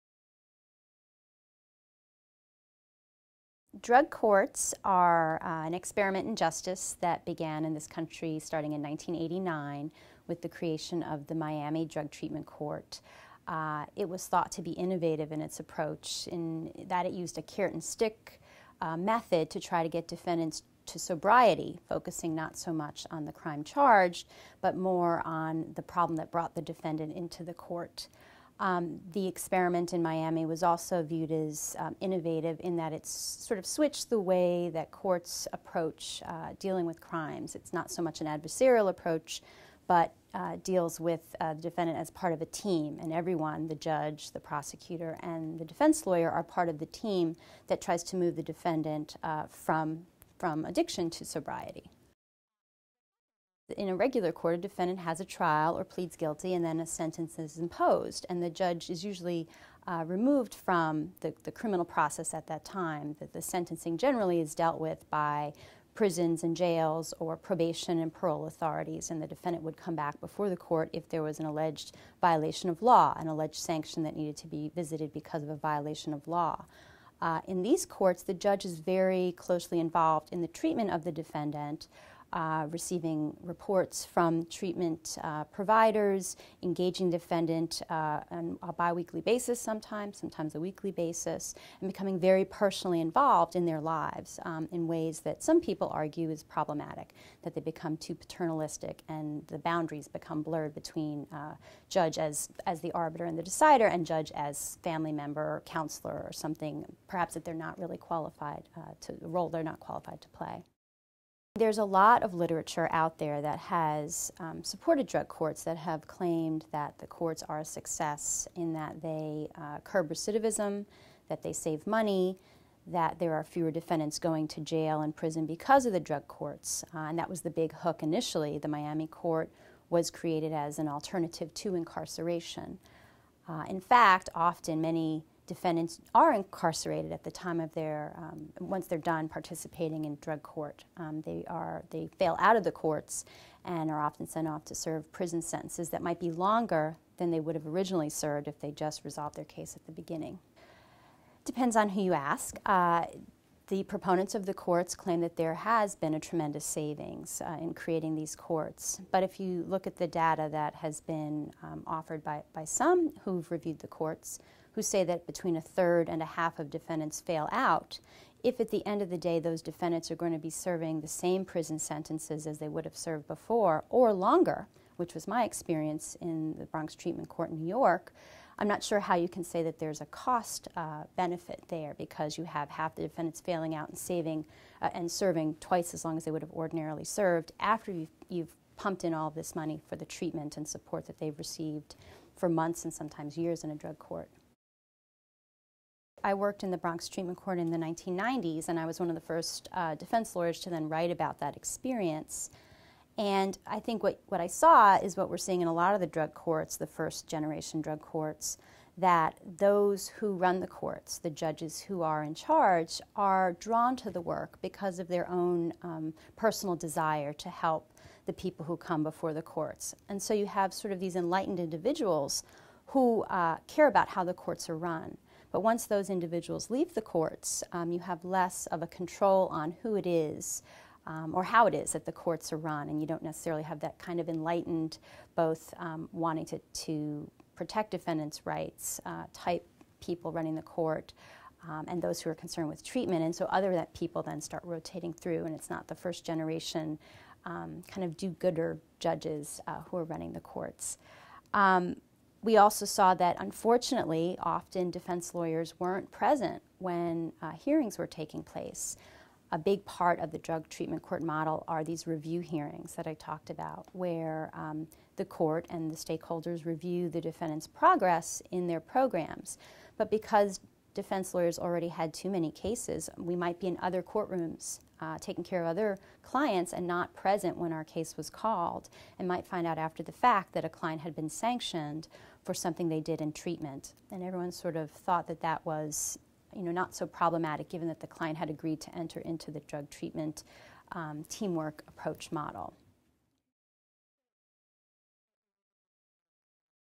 Drug courts are in need of an intervention
Mae Quinn, J.D., problem-solving courts expert and professor of law at Washington University in St. Louis, discusses drug courts, the difference between drug and regular courts as well as the effectiveness of drug courts. She also describes her own experience working in the Bronx Treatment Court. Quinn says that in their zeal to solve problems plaguing our communities, court reformers often overlook important concerns of individual defendants including their rights to due process of law and zealous representation.